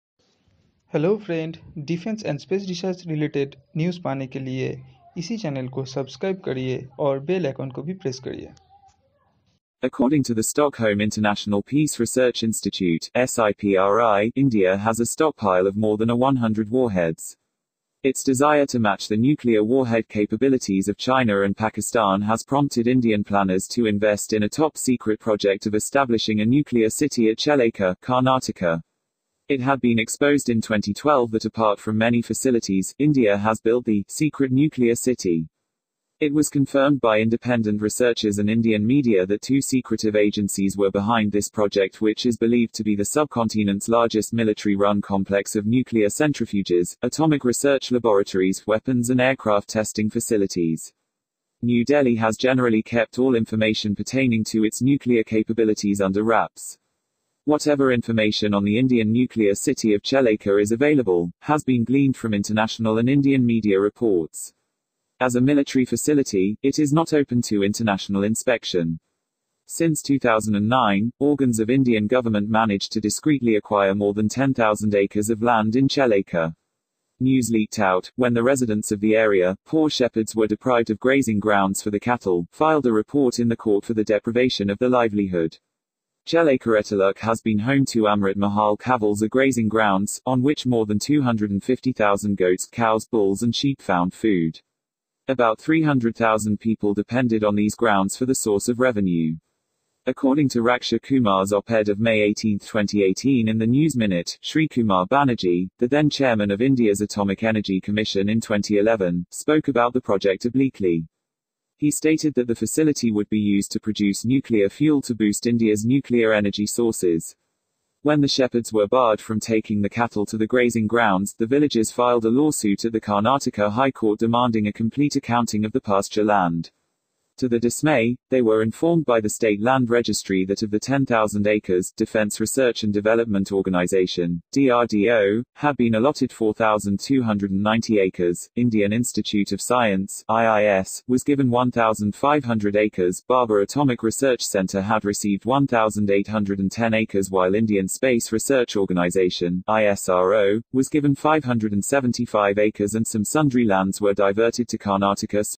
Defence news:Chellakere — India’s Secret Nuclear City - Pakistan Media report.
Chellakere — India’s Secret Nuclear City - Pak Media